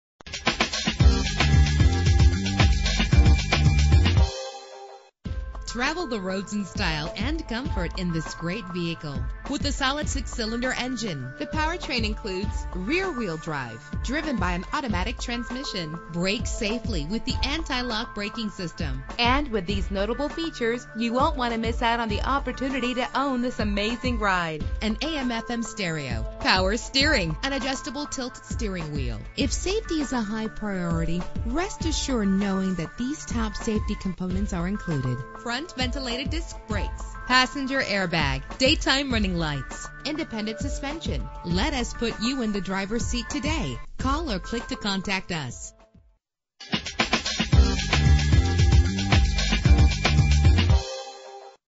2000 Chevrolet Silverado 1500 - White Bear Lake MN
Year: 2000
Make: Chevrolet
Model: Silverado 1500
Trim: WT
Engine: 4.3 liter 6 cylinder 12 valve
Transmission: Automatic
Color: Pewter
Mileage: 122550
Address:
1801 E. County Road F